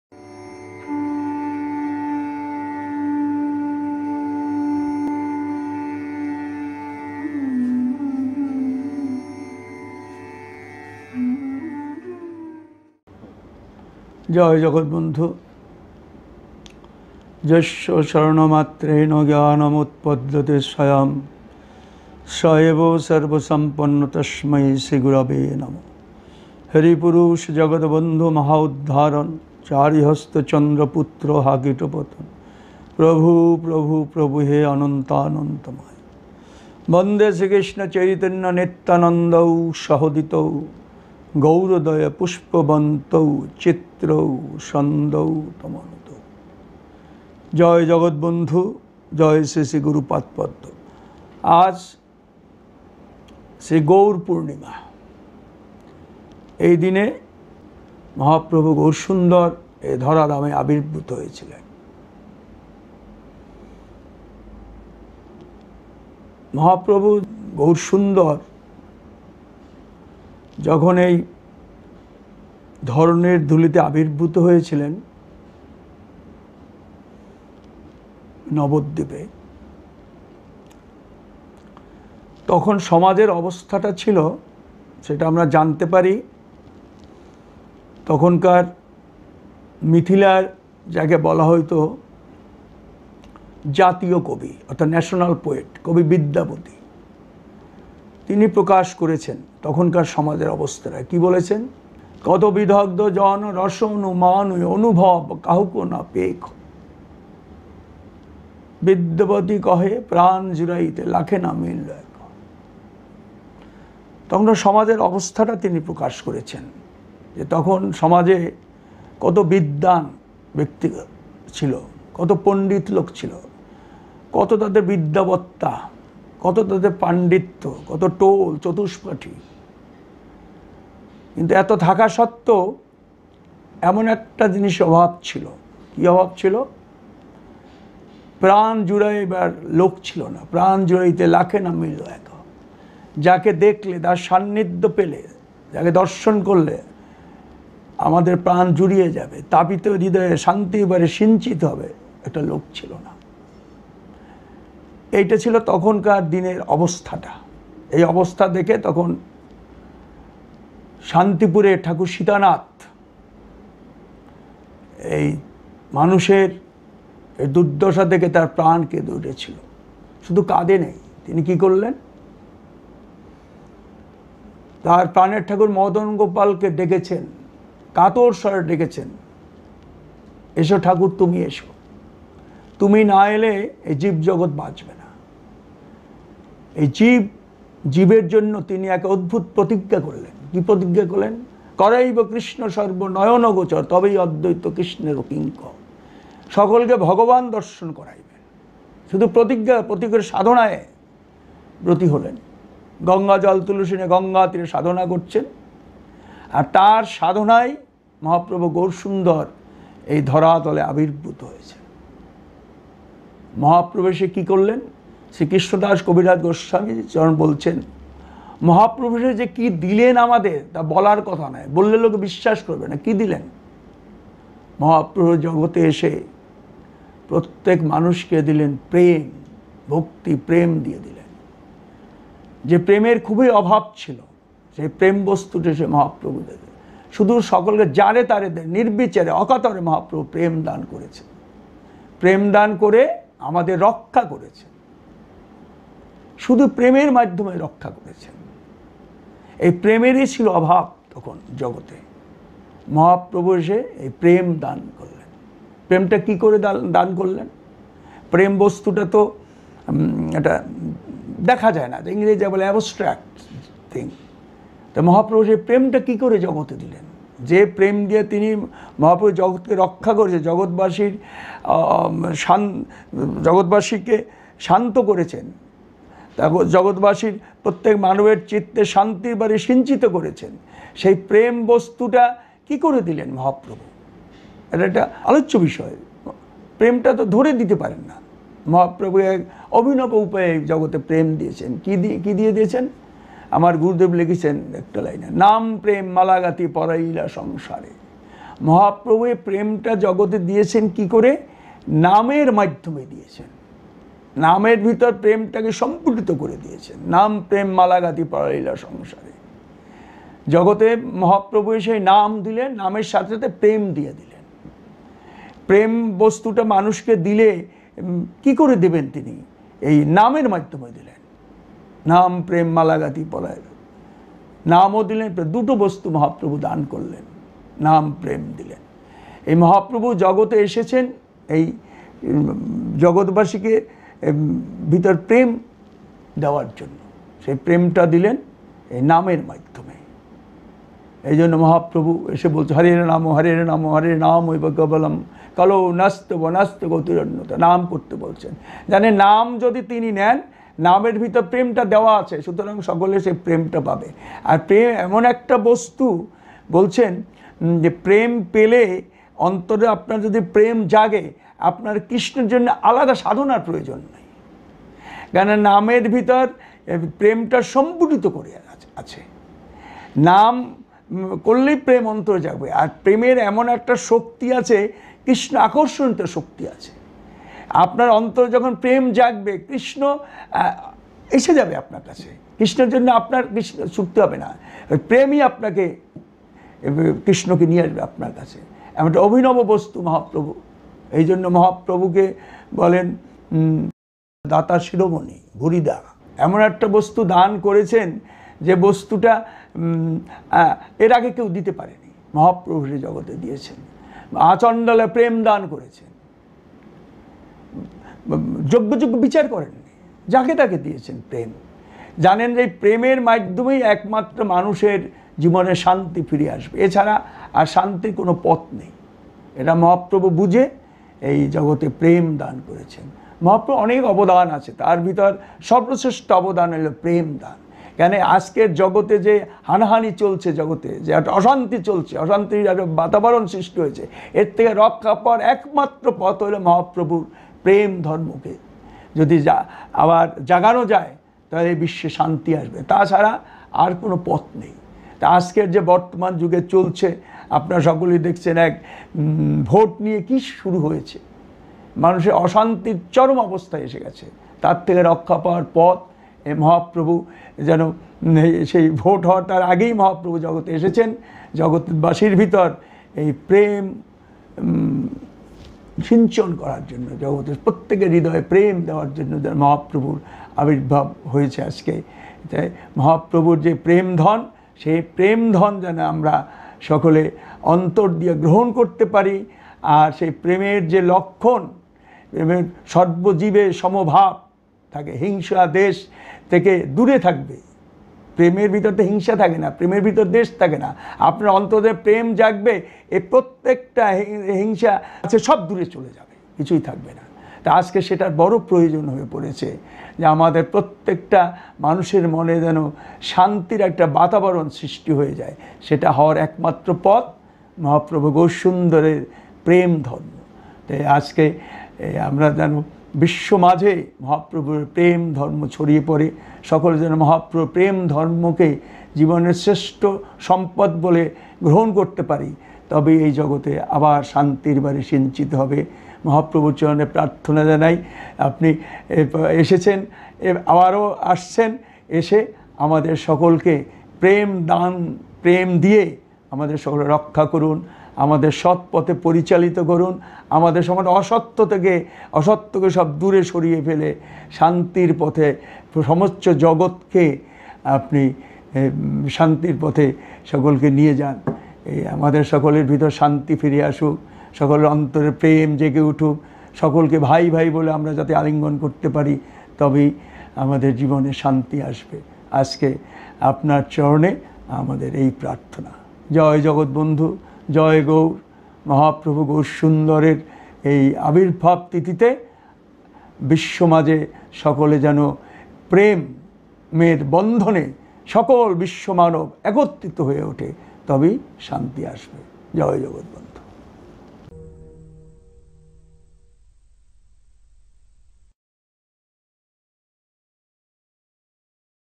Message from Shimat Upasakbandhu Brahmachari on Shri Shri Goura Purnima. 28th March,2021
Shri Shri Goura Purnima Celebrations at Mahanam Angan, Kolkata . 28th March, 2021

Message fromShimat Upasakbandhu Brahmachari President, Mahanam Sampradaya, India